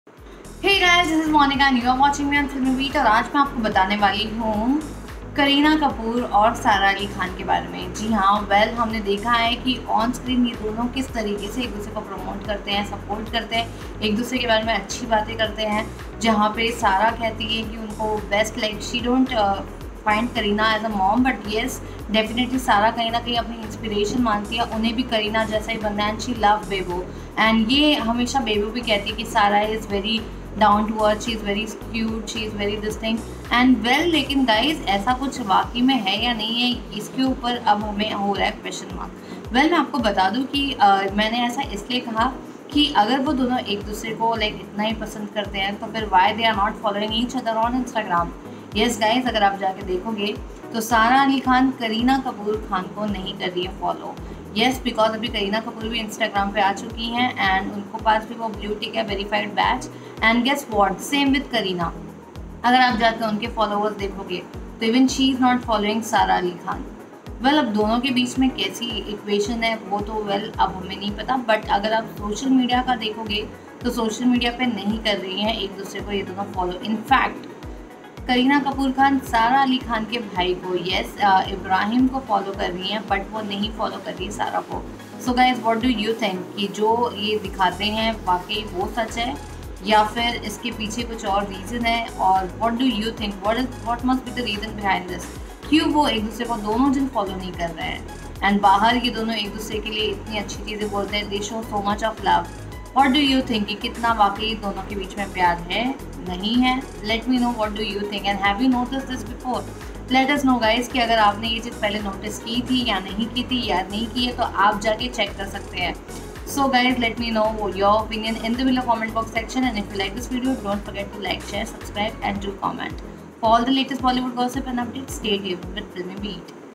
Kareena Kapoor Khan and sara Ali Khan unfollow each other find out where and why?
On camera bebo aka kareena Kapoor becomes sweet and lovey dovey but in reality she don't follow sara Ali Khan and same with sara she shows love for Kareena but doesn't follow her on instagram